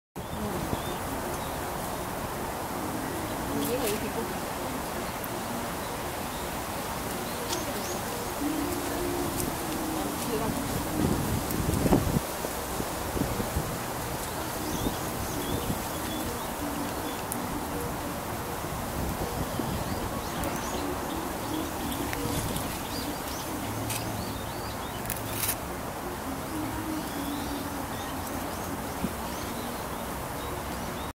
서울숲 기니피그
서울숲 기니피그 사육장에 있는 기니피그

촬영일시 : 2013년11월11일
촬영장소 : 서울숲
카메라 : Nikon 1 J1